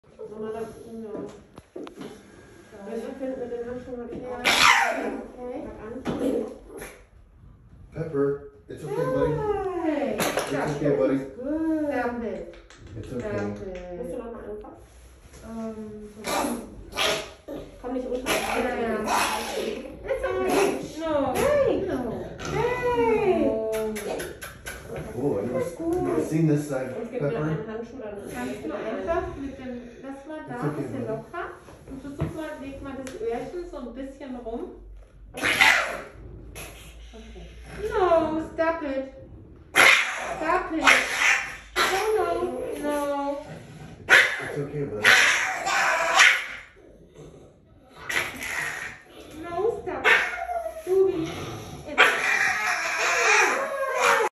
PUG went to the Vet clinic REACTION | DRAMATIC PUG | VET VISIT | Pepperthewandererpug
Pepper hates being touch in the ear.